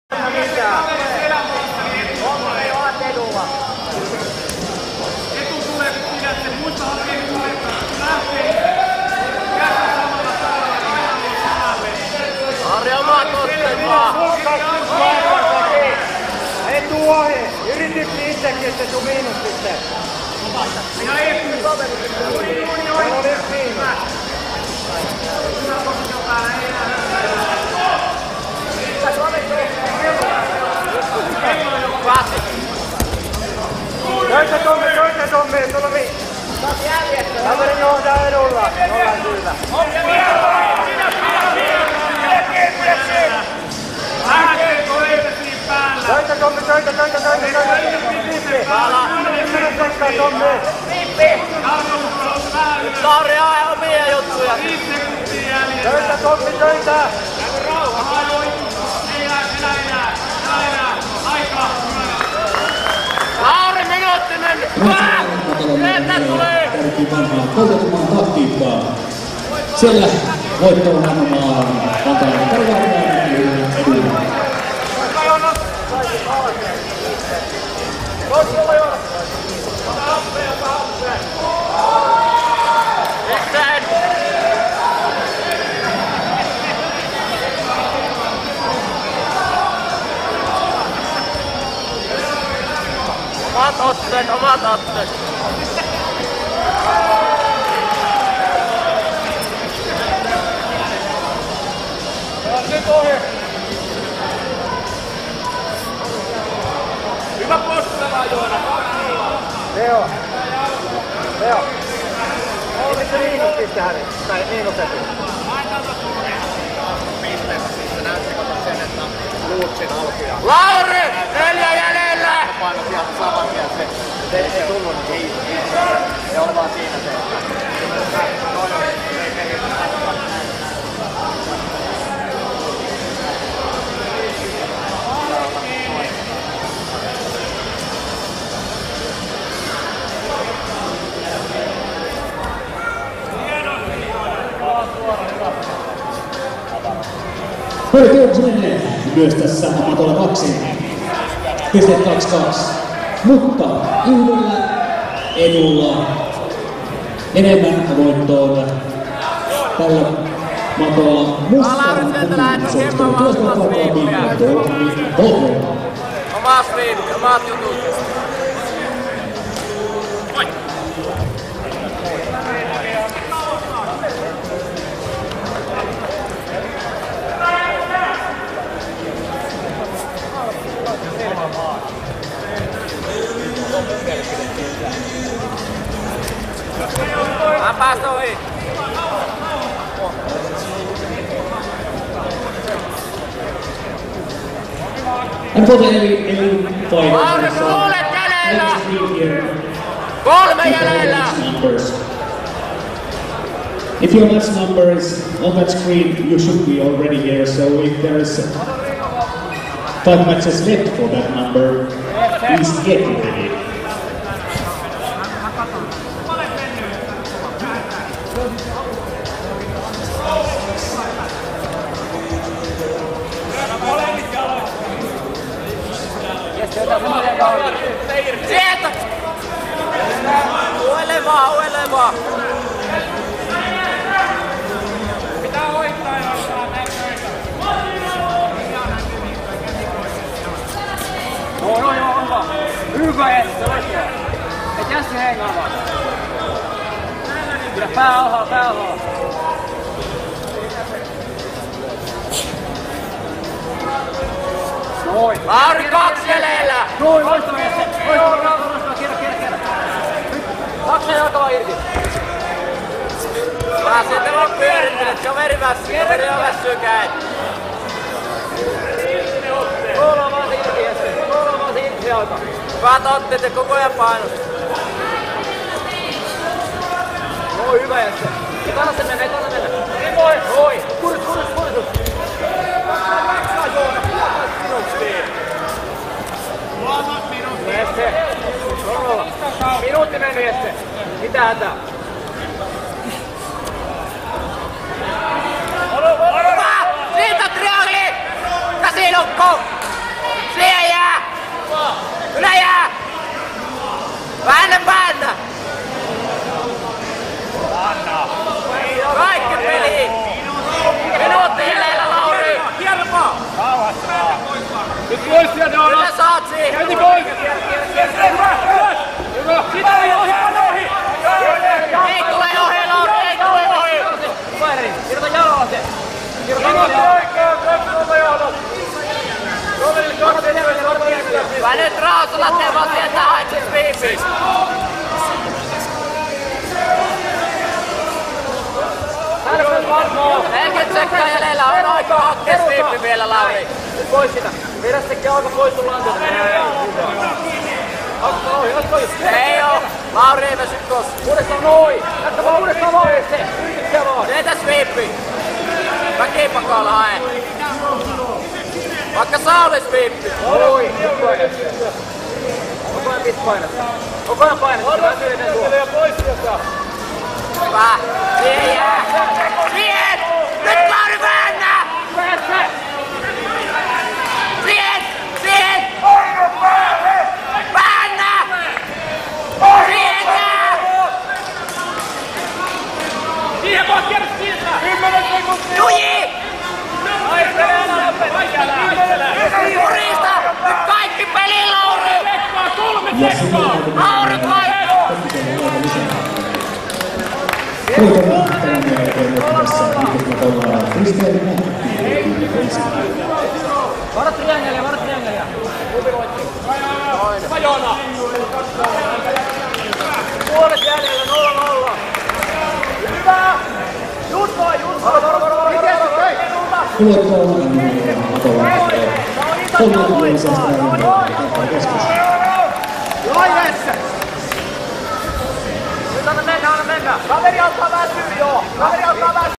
Finnish BJJ Open Day 1 - Fight 65.mp4
Video of a fight at the Finnish BJJ Open 2012